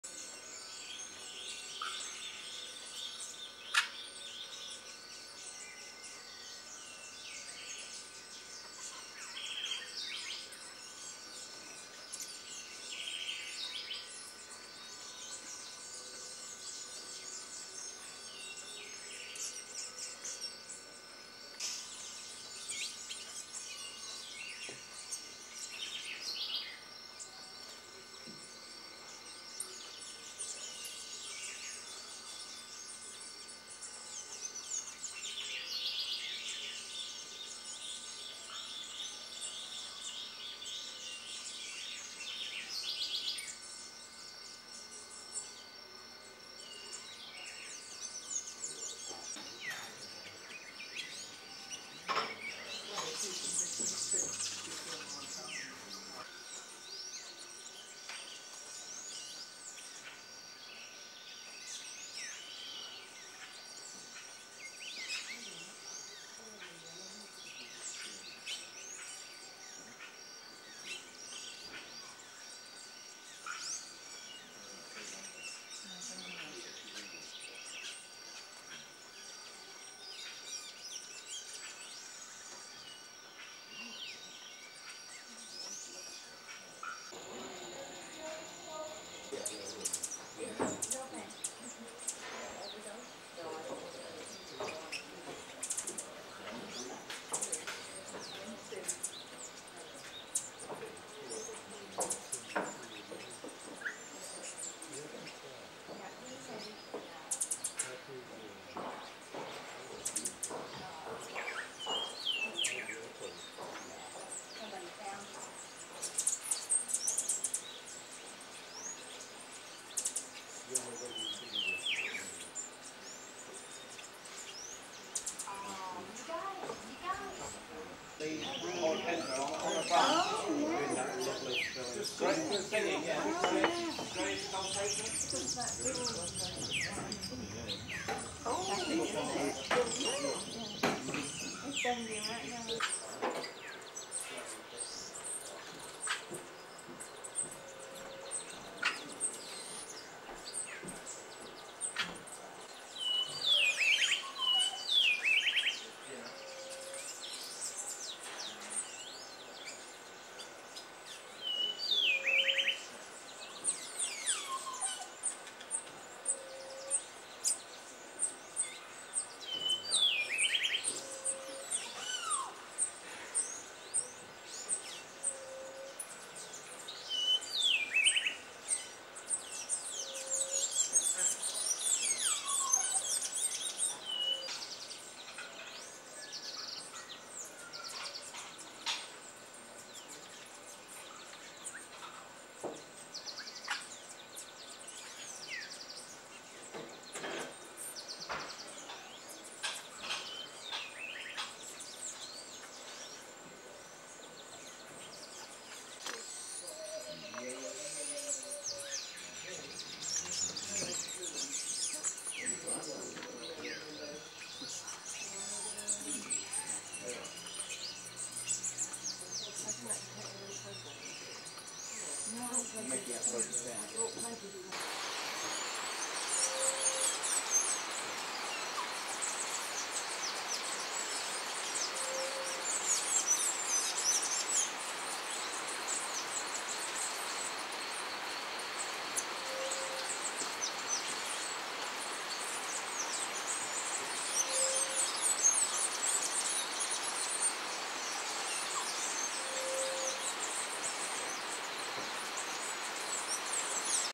Crested oropendolas, Asa Wright Nature Centre, Trinidad
Crested oropendolas, Asa Wright Nature Centre, Trinidad - 18th-22nd April 1999
These birds are probably the most noticeable at Asa Wright. Their distinctive hanging nests are in the trees in front of the verandah and they can be seen constantly. Their bizarre head-down display and call is unforgettable.